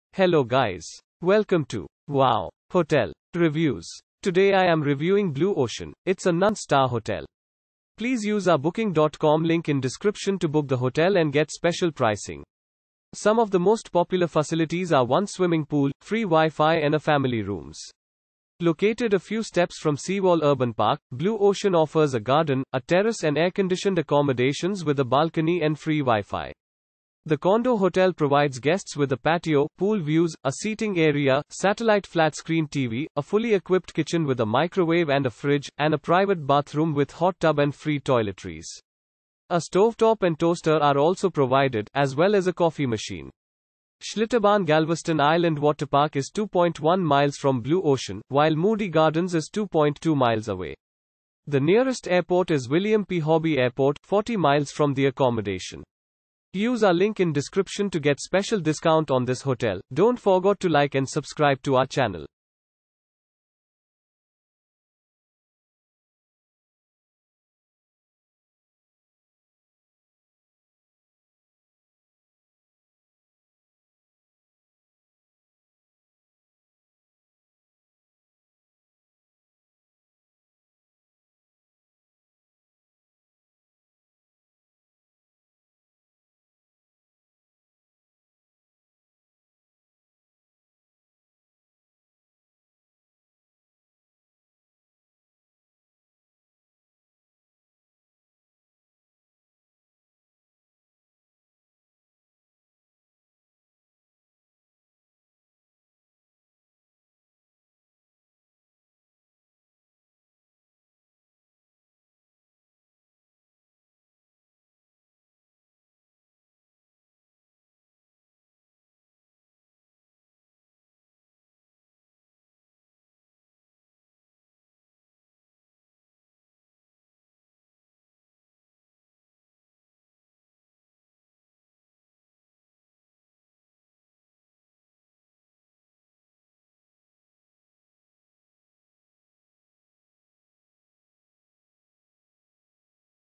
Blue Ocean Review - Galveston , United States of America
Blue Ocean

Located a few steps from Seawall Urban Park, Blue Ocean offers a garden, a terrace and air-conditioned accommodations with a balcony and free WiFi.
The condo hotel provides guests with a patio, pool views, a seating area, satellite flat-screen TV, a fully equipped kitchen with a microwave and a fridge, and a private bathroom with hot tub and free toiletries. A stovetop and toaster are also provided, as well as a coffee machine.
Schlitterbahn Galveston Island Waterpark is 2.1 miles from Blue Ocean, while Moody Gardens is 2.2 miles away. The nearest airport is William P. Hobby Airport, 40 miles from the accommodation.